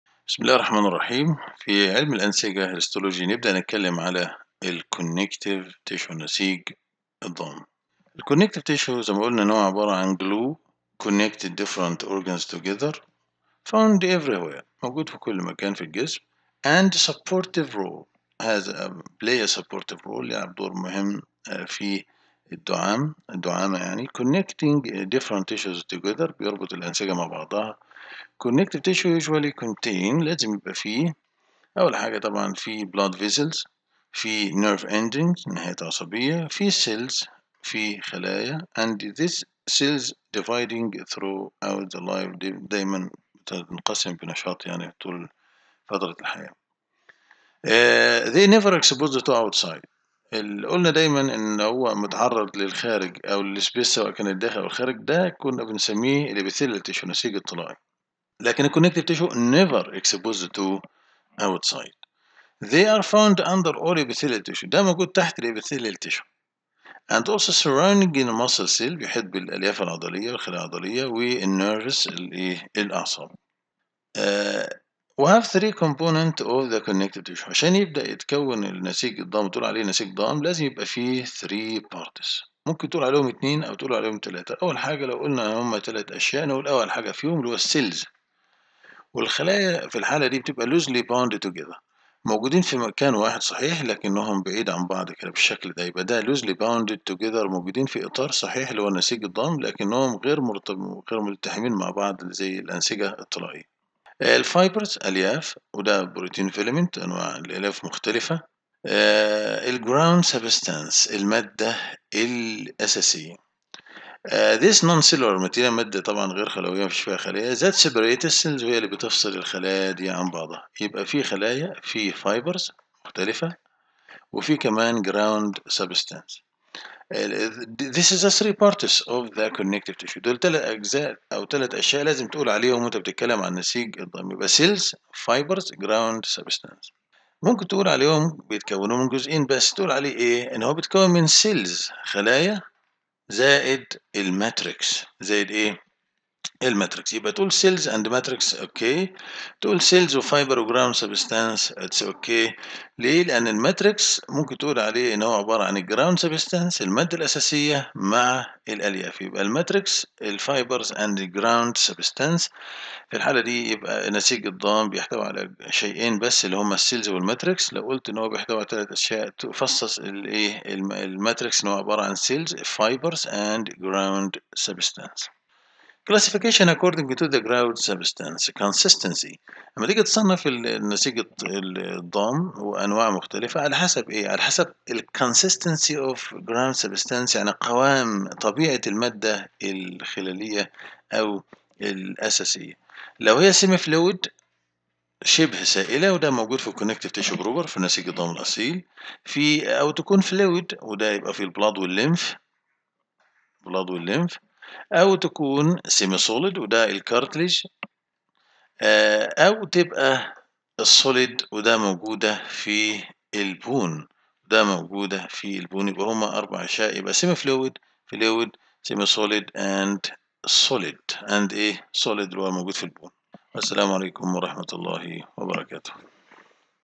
علم الحيوان Zoology | الوحدة 6 | Connective tissues
الوحدة 6 : Histology
الدرس 2 : Connective tissues
1- Components of connective tissue
2- Classification of C T according to the
     consistency of ground substance